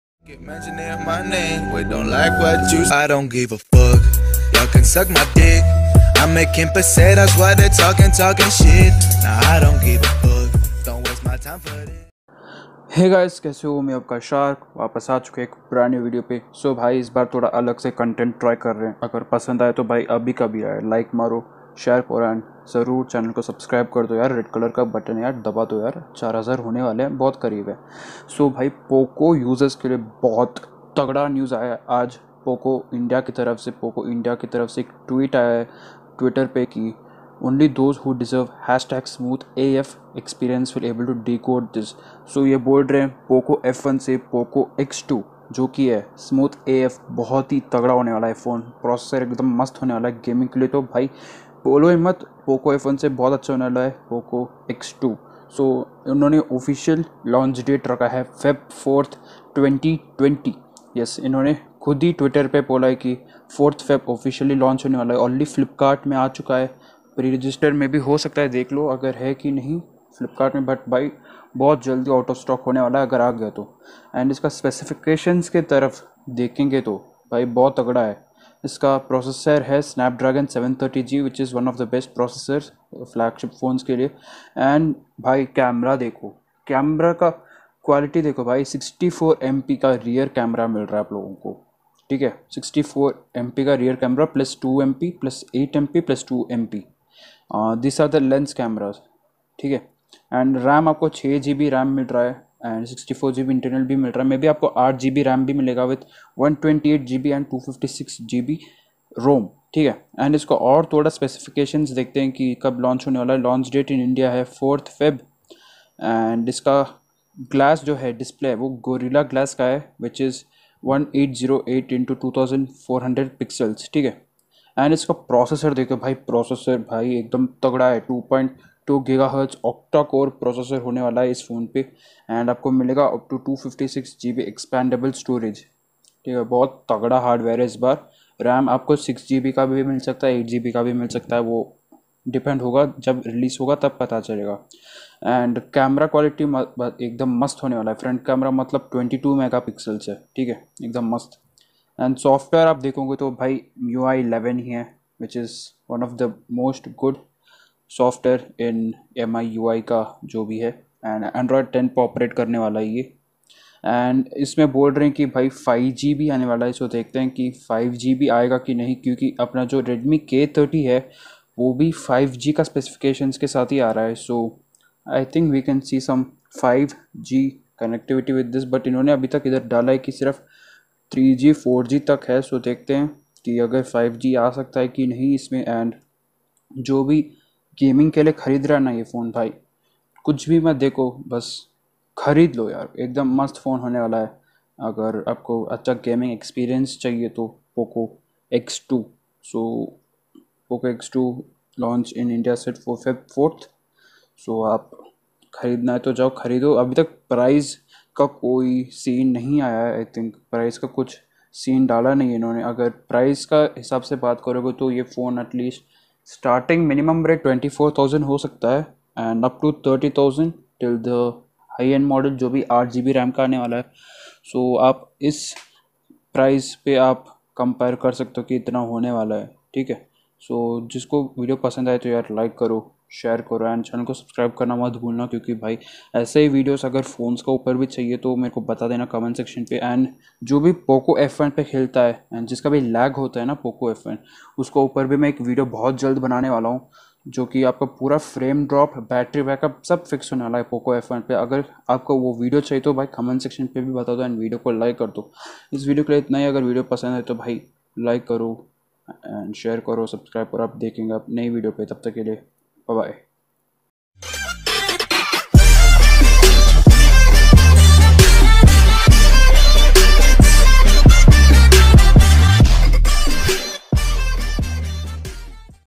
Poco Is Back | Poco X2 SmoothAF | Specs | Launch Date | Gaming??
Poco X2 is officially releasing on 4th Feb 2020
In Flipkart store
Go check it out
Poco X2

Pubg ke like it's one of the best phone

If you want video on how to reduce lag In Poco F1
Let me know in comments
Am working on it